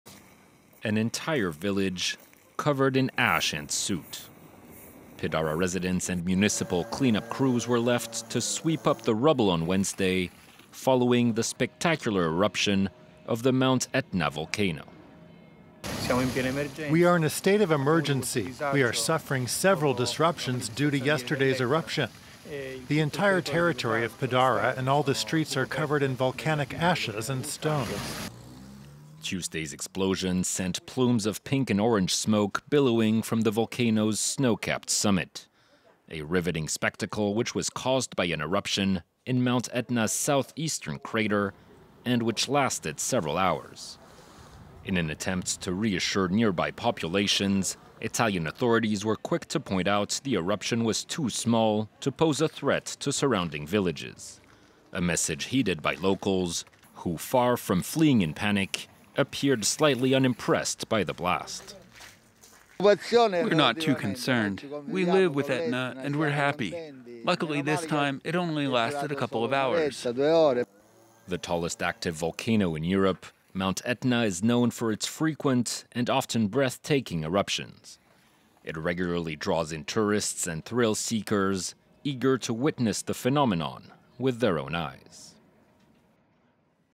Mount Etna spews smoke and ashes in spectacular new eruption
Mount Etna, one of the world's most active volcanoes, belched smoke and ashes in a new eruption on Tuesday, but Italian authorities said it posed no danger to the surrounding villages. Images showed a spectacular rose-coloured plume of ashes above the snow-capped summit, but the cloud had largely dissipated by nightfall, while lava flows continued to glow.